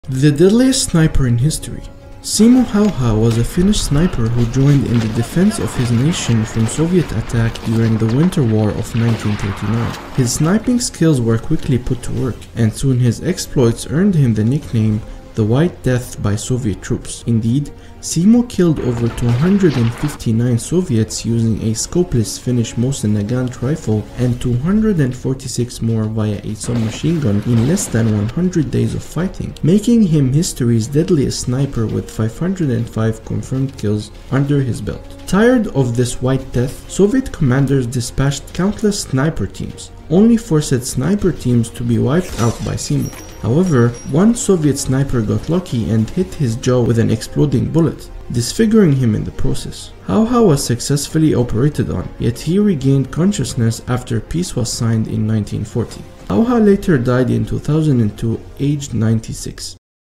The White Death | Rapid History
Simo Häyhä was a Finnish sniper who joined in defending his nation from the Soviet Union attack during the Winter War of 1939.

His unrivaled exploits earned him the legendary nickname "the white death" by Soviet troops, provoking the ire of Soviet commanders.

He ended the war by becoming the single deadliest sniper in history, with 505 confirmed kills.